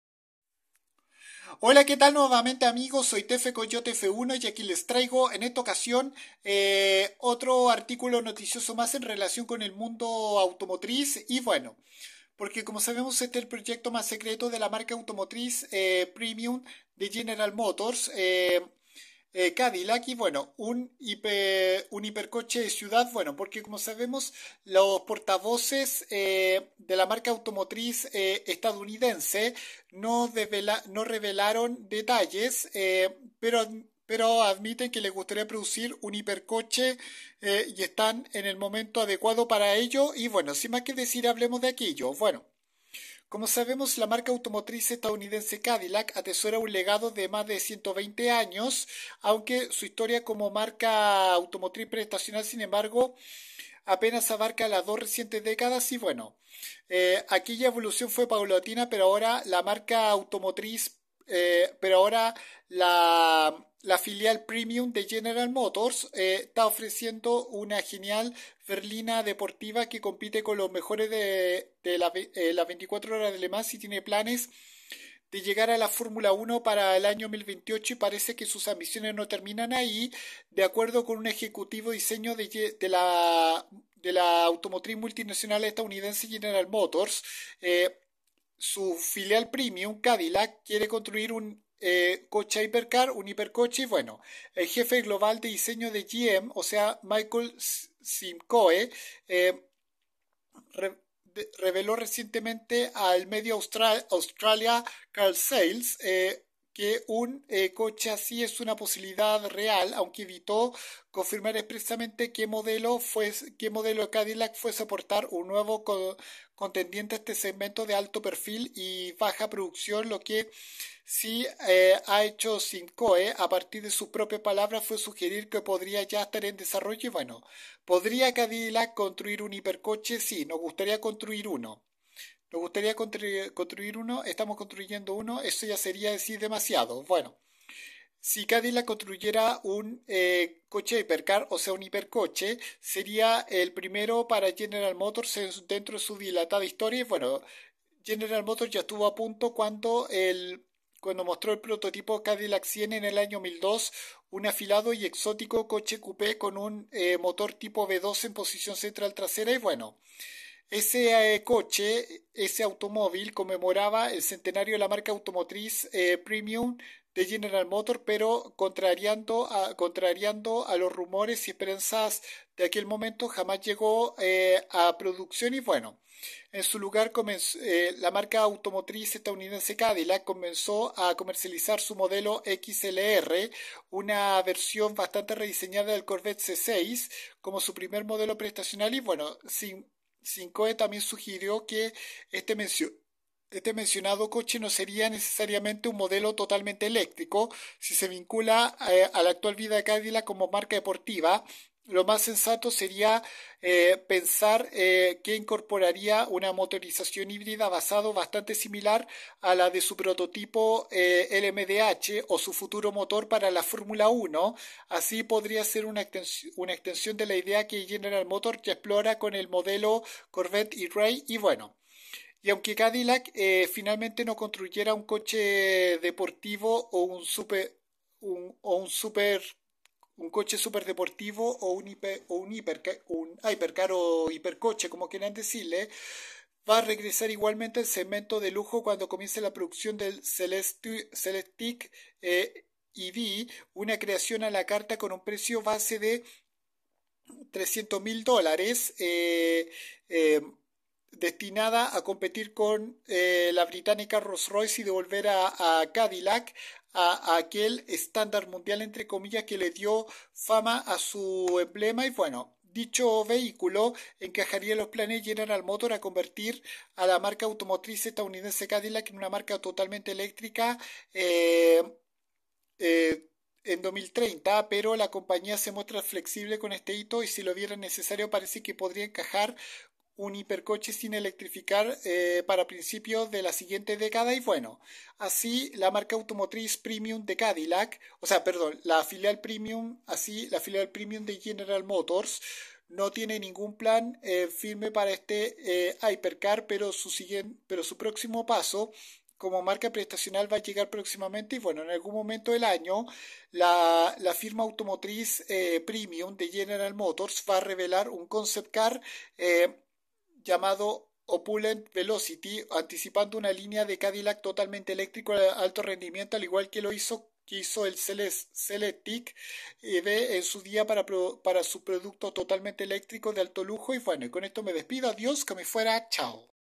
Cadillac presenta su muy secreto proyecto
Se dice que la filial premium de General Motors podría estar preparando un hipercoche